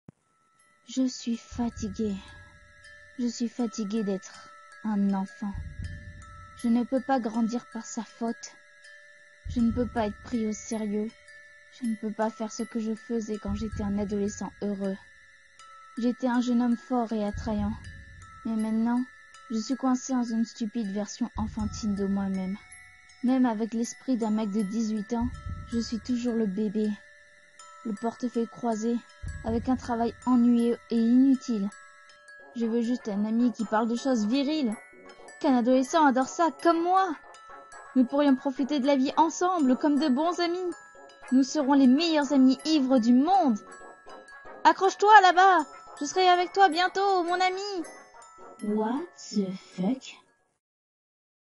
[Undertale FR] Underverse ~ Cross Chara veux grandir ~
~ Lieu magique en dessous ~

Heyyyy p'tit comic un peu bizarre sur cross chara, le pauvre il est coincé dans un corp d'enfant alors qu'ils pourrais faire bien mieux ! mais wait ... est-ce que ça veux dire qu'il a déjà étais grand ? il y a de quoi se poser des questions, non ?

- Toutes les voix sont faites par moi et le comic ne m'appartient pas, artiste: Jakei

// Réseaux socieaux \\

J'espère que ça vous aura plus !